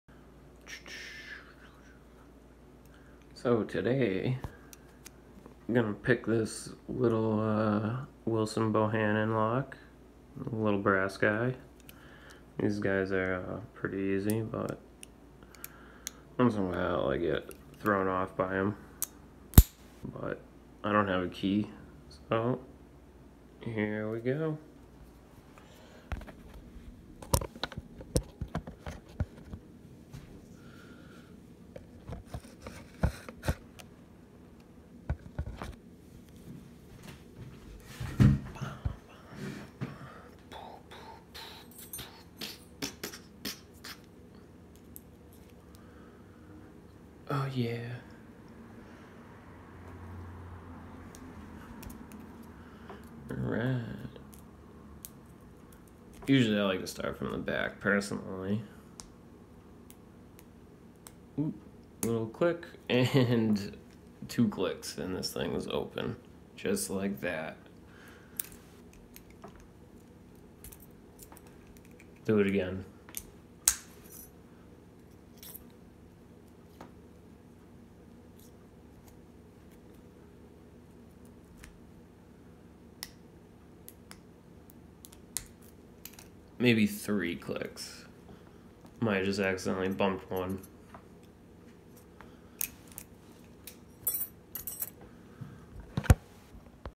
Picking a WB Brass lock. Opens in seconds. Wilson Bohannan padlock picked.
I make all my own tools.